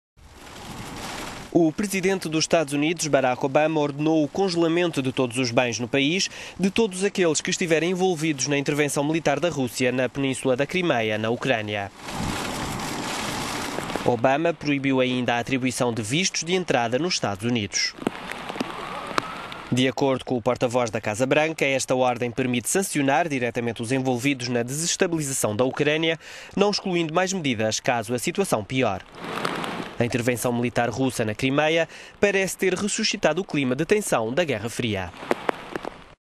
Ucrânia: EUA impõem sanções a envolvidos na intervenção russa na Crimeia
O presidente dos Estados Unidos, Barack Obama, ordenou o congelamento de todos os bens, no país, de todos aqueles que estiverem envolvidos na intervenção militar da Rússia na península da Crimeia, na Ucrânia.

Obama proibiu, ainda, a atribuição de vistos de entrada nos Estados Unidos.

De acordo com o porta-voz da Casa Branca, "esta ordem permite sancionar, diretamente, os envolvidos na desestabilização da Ucrânia", não excluindo mais medidas, caso a situação piore.

A intervenção...